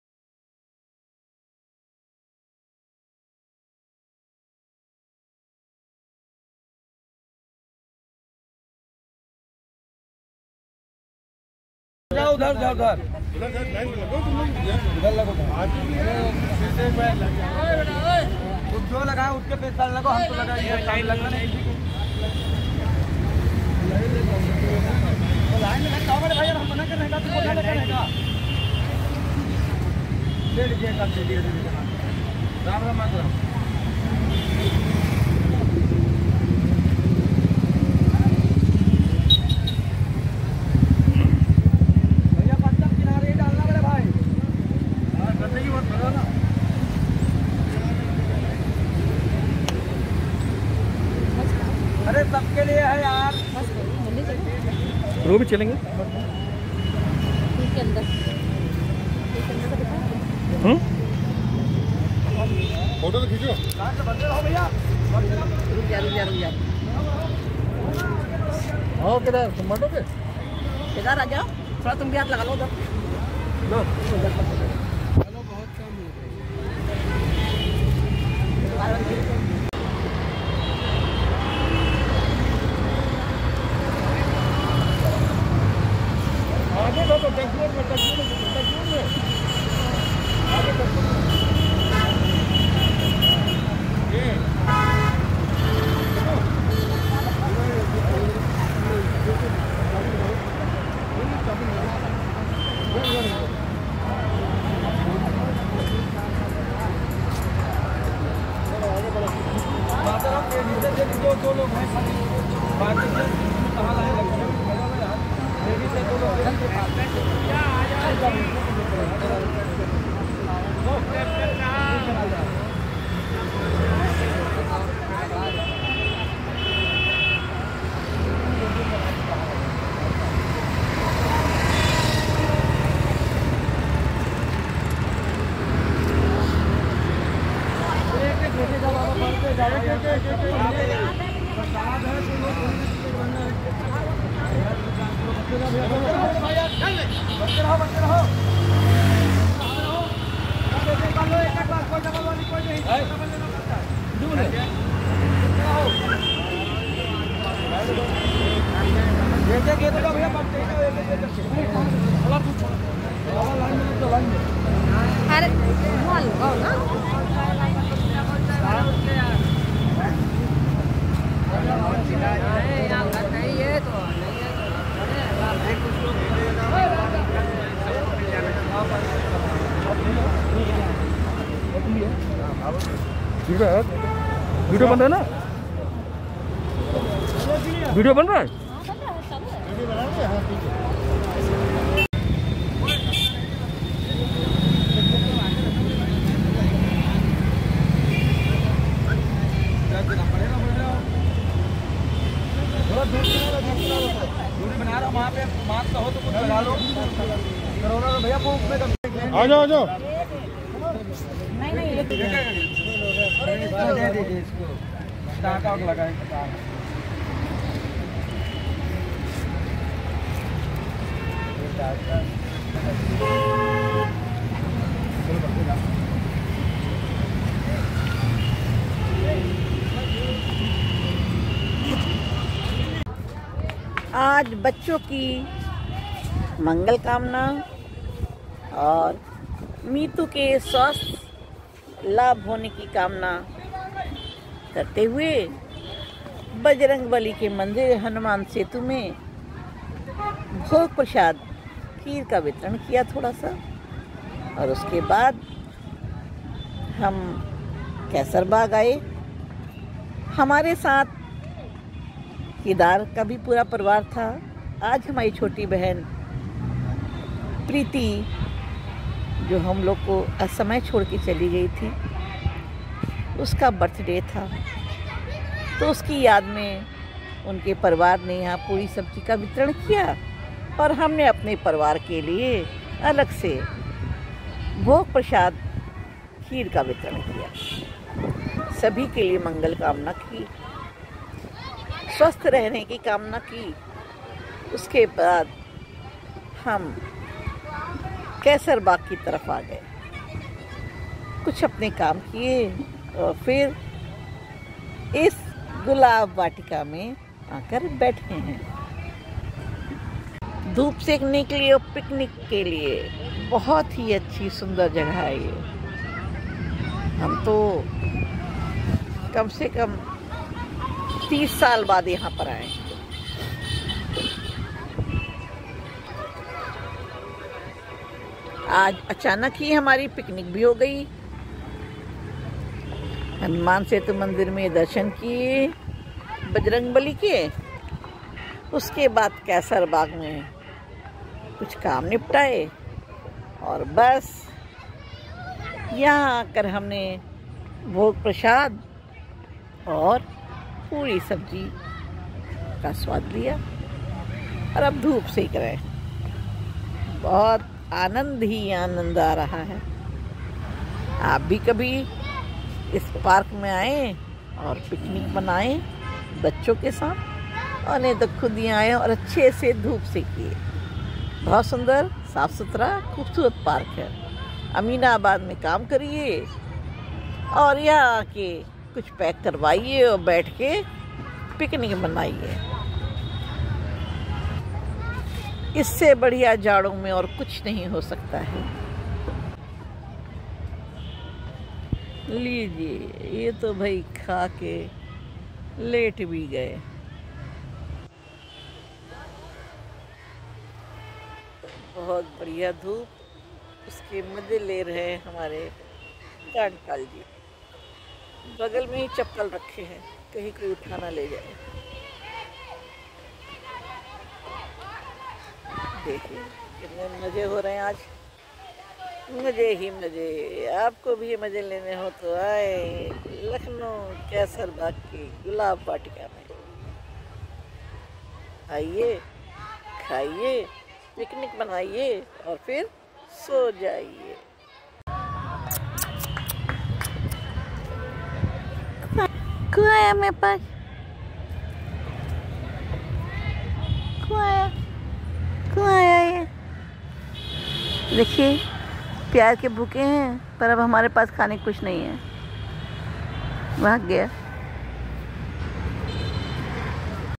30 -2 -2022 ko hanuman Setu Mandir me bhog Prasaad kheer ka vitran
bacchon or hubby ki mangal  kaamna ke liye bhog Prasaad kheer ka vitran or gulab Vatika me guzare kuch pal